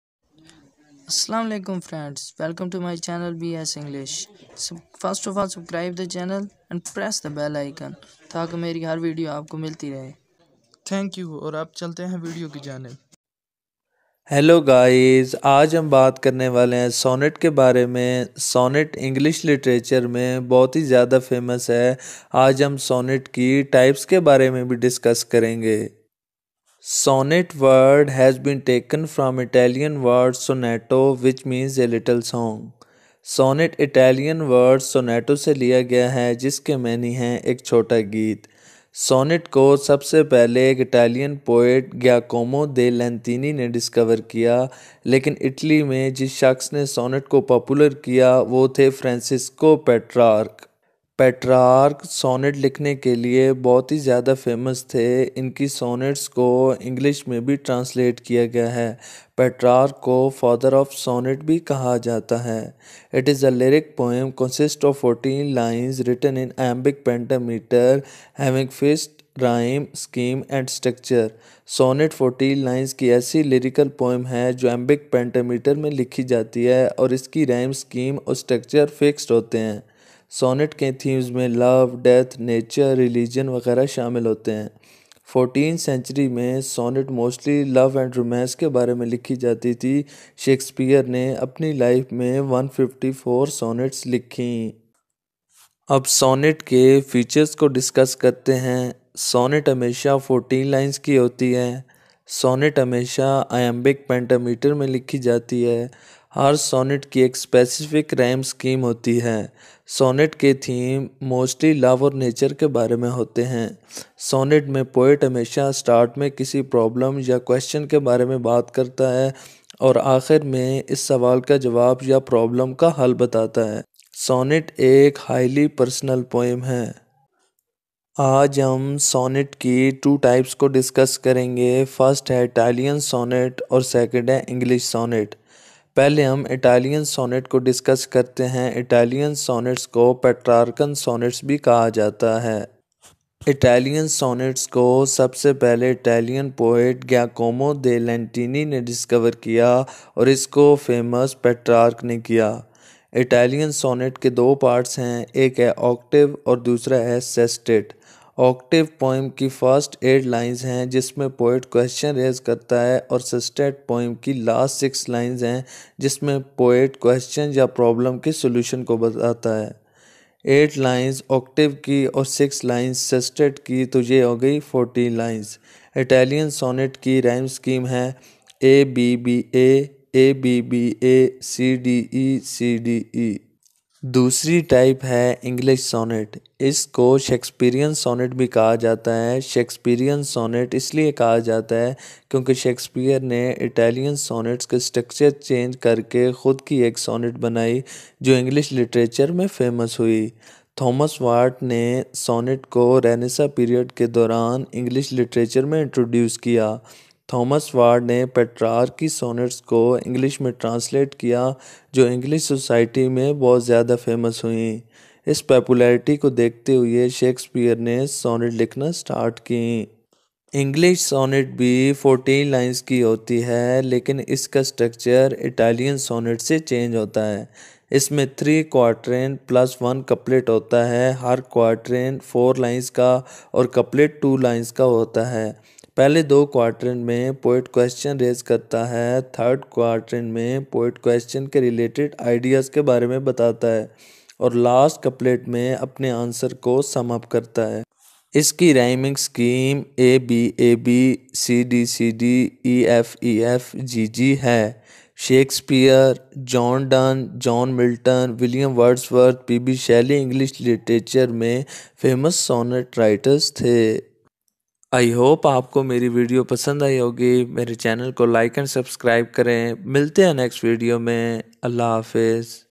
Sonnet and its types explained in Hindi Urdu.
A sonnet (pronounced son-it) is a fourteen line poem with a fixed rhyme scheme. Often, sonnets use iambic pentameter: five sets of unstressed syllables followed by stressed syllables for a ten-syllable line. Sonnets were invented by the Italian poet Giacomo da Lentini during the 1200s.